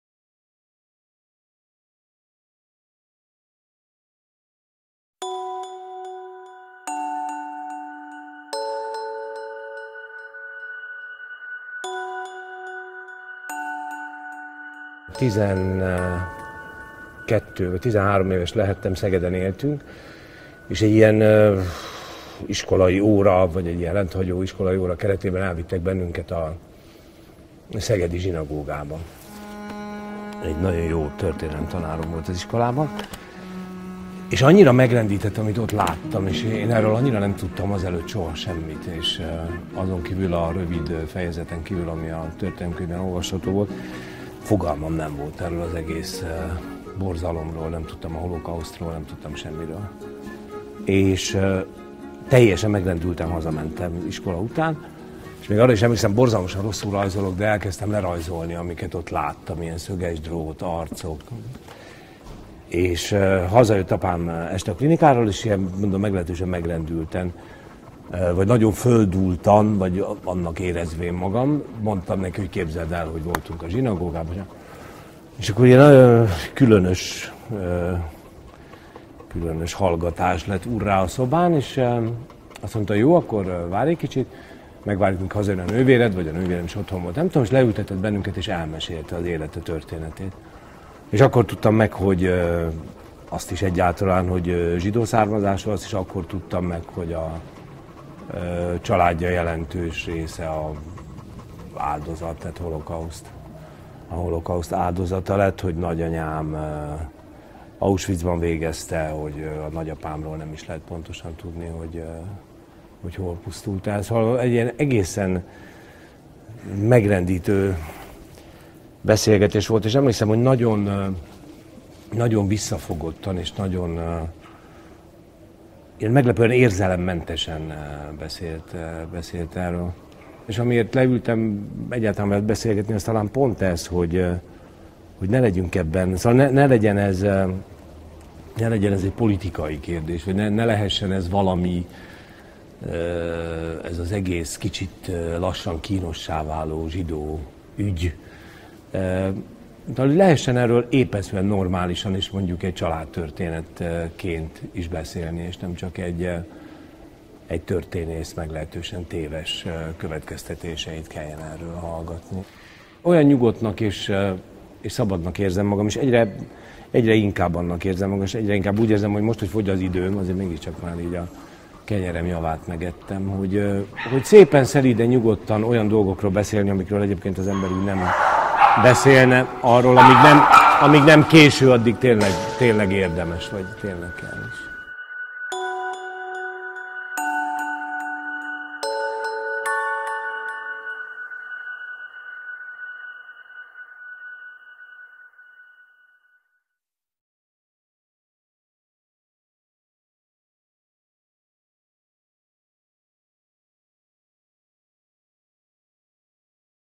Zsidó mesék - Kulka János
Az Egységes Magyarországi Izraelita Hitközség (EMIH) felkérésére Gárdos Péter filmrendező a csillagos házakban készített rövid videókat ismert és elismert művészekkel, közéleti és tudományos személyiségekkel arról, hogy mit jelent számukra a zsidóság. A több mint 50 megszólaló többsége nem zsidó vallású, identitású.

A fővárosban és több vidéki helyszínen is bemutatni tervezett közel háromórányi a Youtube-on is megtekinthető videók célja a zsidókkal kapcsolatos pozitív gondolatok, élmények megmutatása.

A projekt a Civil Alap 2014 támogatásával és a Tett és védelem alapítvány közreműködésével valósult meg.